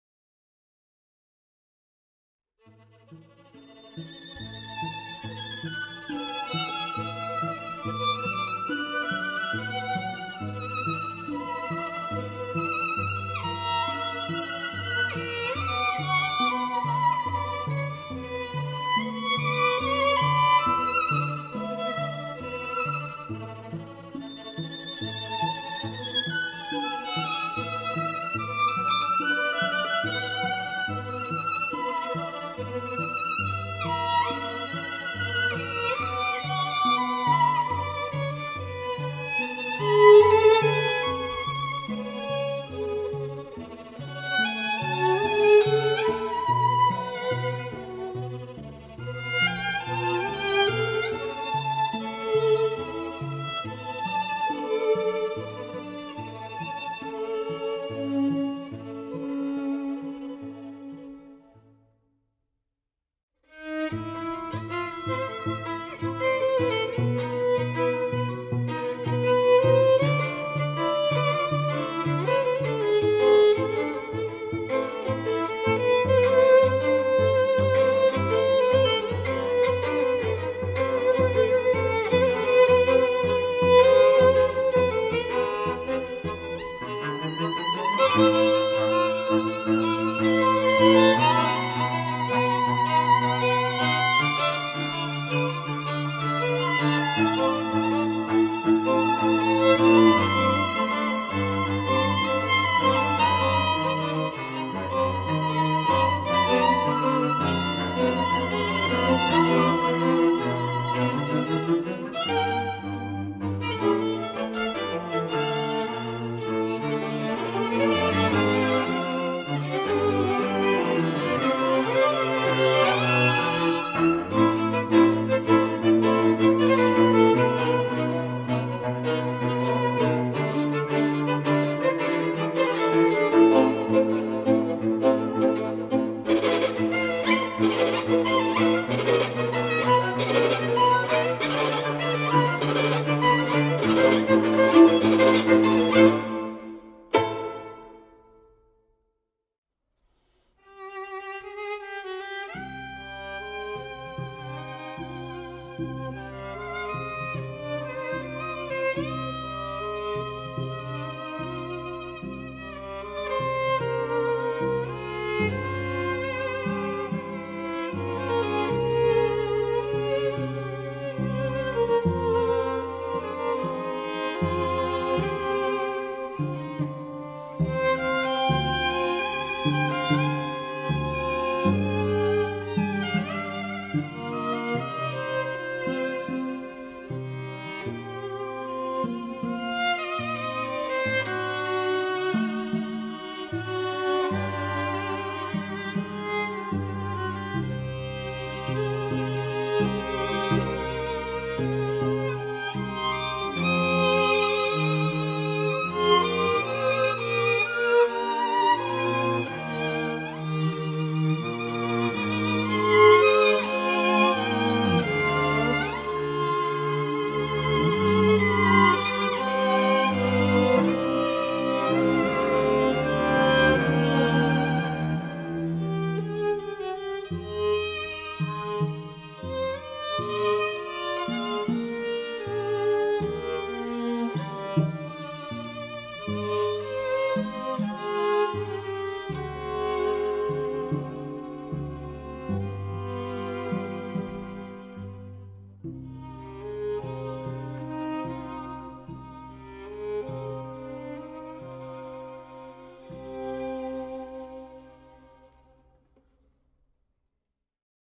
Струнный квартет "Глория"
Квартет "Глория" создан, чтобы очаровывать самую взыскательную публику! Это живое, красивое и волнующее музыкальное сопровождение любого мероприятия!